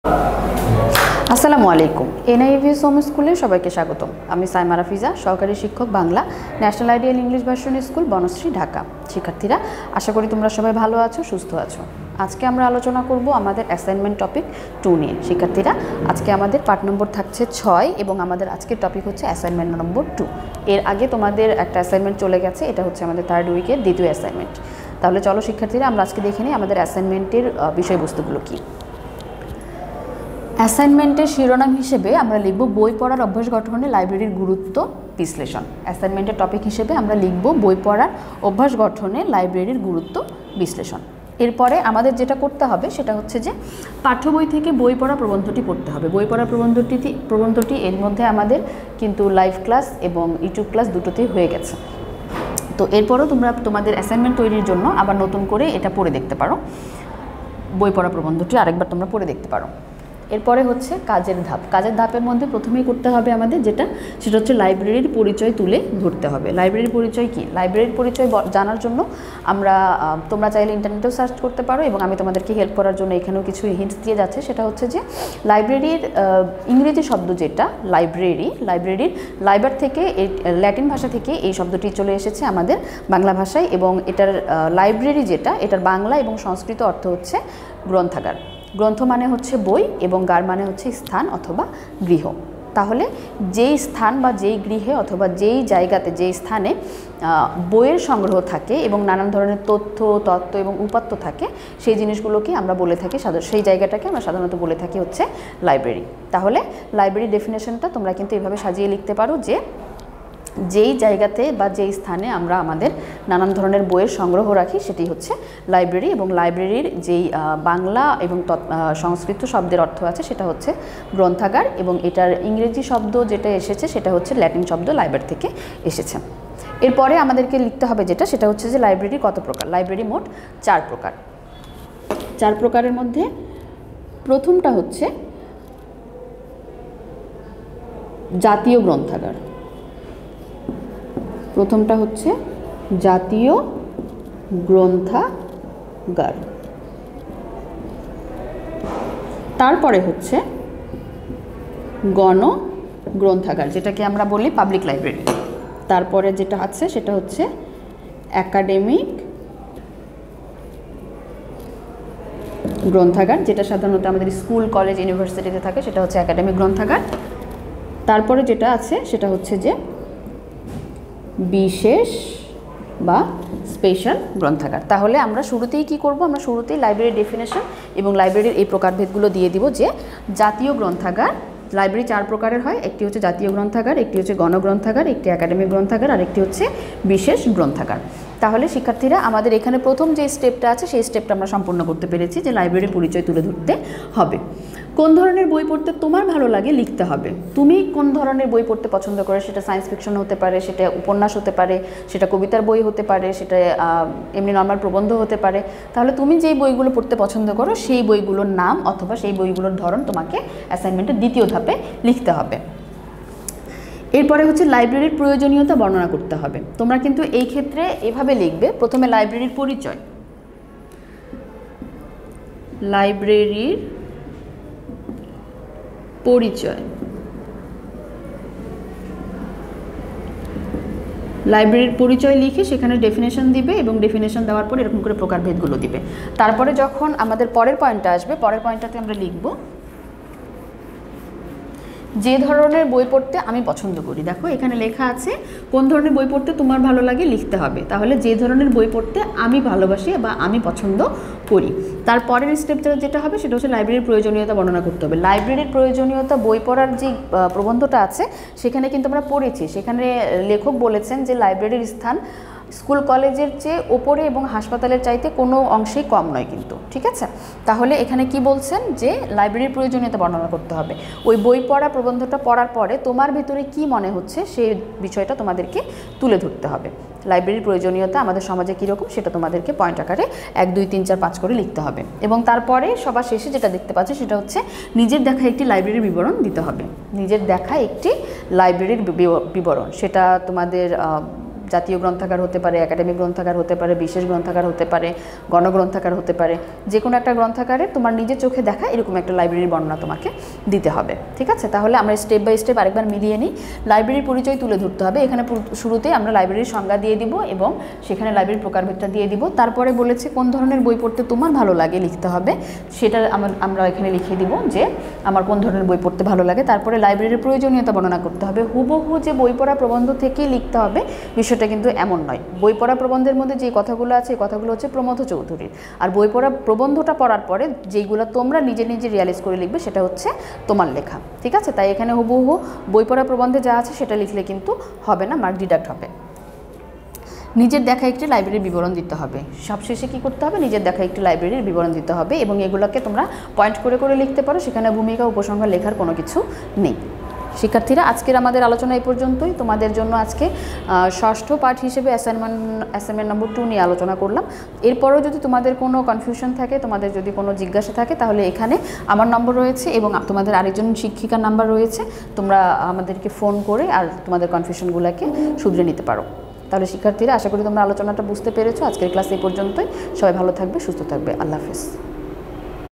শ্রেণিঃ নবম, বিষয়ঃ বাংলা ১ম পত্র (পাঠ- ০৭), আলোচ্য বিষয়ঃ অ্যাসাইনমেন্ট-২
অর্ধ-বার্ষিক-২০২২
শ্রেণিঃ নবম
বিষয়ঃ বাংলা ১ম পত্র (পাঠ- ০৭)
আলোচ্য বিষয়ঃ অ্যাসাইনমেন্ট-২
শিক্ষকঃ সায়মা রাফিজা